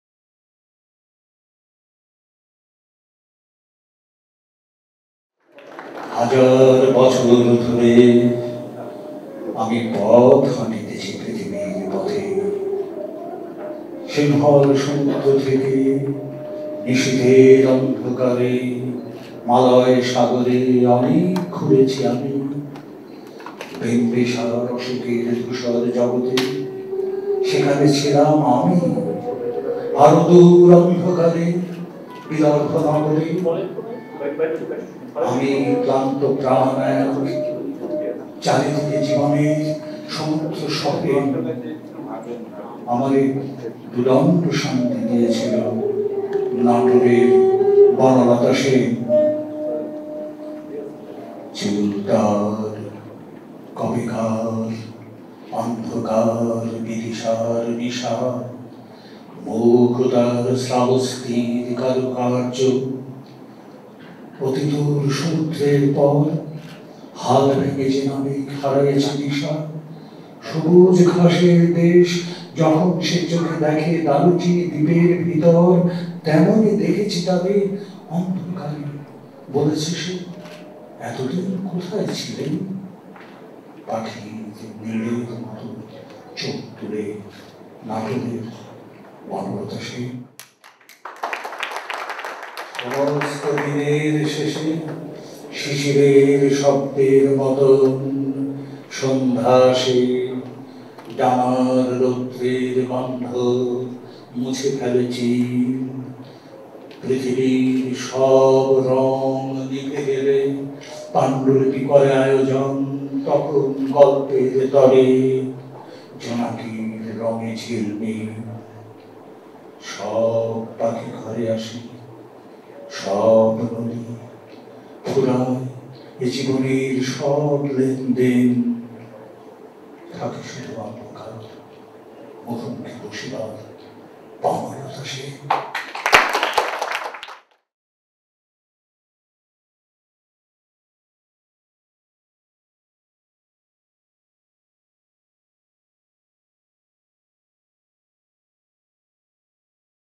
Remembering Syed Shamsul Haque on his 8th death anniversary I Banalata Sen I Jibanananda Das
Syed Shamsul Haque (1935–2016) was born in Kurigram, in northern Bangladesh. One of the most important and versatile writers in Bengali, Haque’s poetry, fiction, plays, and essays, have influenced generations of poets and writers. He received the Bangla Academy Literary Award in 1966, the Ekushey Padak in 1984 and the Independence Day Award (Bangladesh’s highest civilian award) in 2000 for his contribution to Bangla literature. His notable works include Payer Awaj Pawa Jai, Nishiddho Loban, Khelaram Khele Ja, Neel Dongshon and Mrigoya.

Haque was the Director-General of Bengal Foundation in the early nineties. We would like to remember Syed Shamsul Haque on his eighth death anniversary and celebrate his monumental presence in Bengali literature. The video clip is taken from a longer presentation by Syed Haque in 2011 at Bengal Shilpalay.